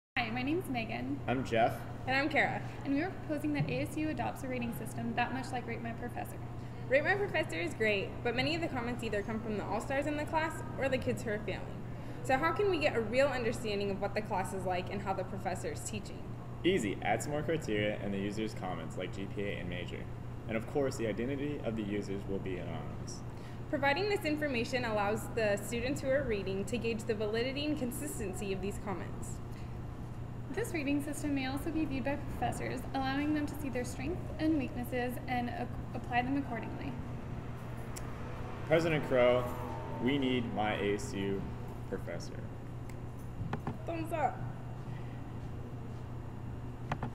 myASUratemyprofessor
College students frequently utilize the website ratemyprofessor.com. This website allows students to rate teachers and provide a brief description of their experience in a specific class. While the idea of this site is valid, there are flaws that can easily be corrected, enhancing the validity and reliability of the ratings.